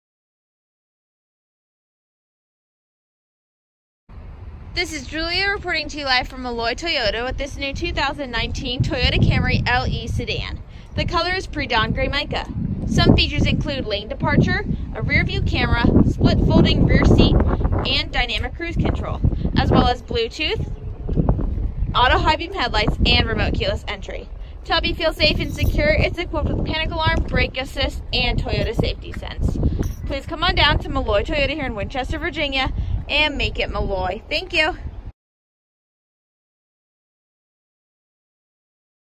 New 2019 Toyota Camry LE Sedan CK336
New 2019 Toyota Camry LE Sedan

Exterior color: Predawn Gray Mica
Interior color: Ash
Lane departure, emergency communication system, rearview camera, split folding rear seat, dynamic cruise control, bluetooth, auto high beam headlights, remote keyless entry, panic alarm, brake assist, traction control, Toyota Safety Sense, 28mpg city, 39mpg highway, 17inch wheels